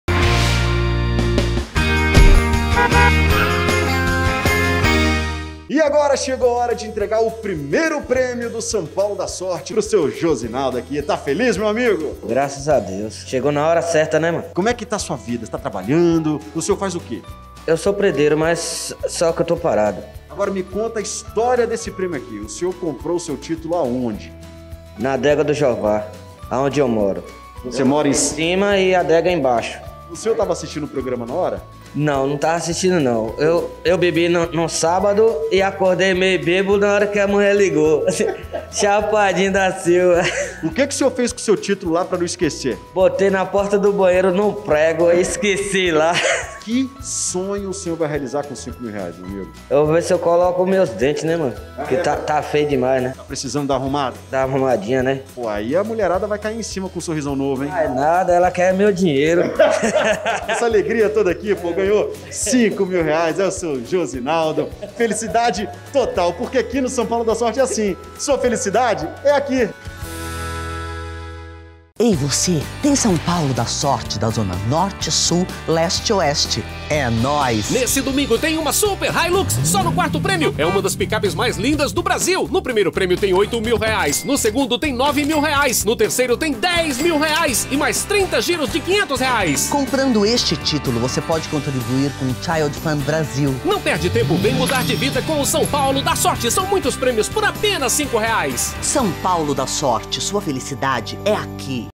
ENTREGA DO 1° PRÊMIO, SÃO PAULO DÁ SORTE DIA 28.02.2021
O Josenaldo dos Santos ganhou sozinho o 1º Prêmio do domingo, dia 21/02/2021. Confira!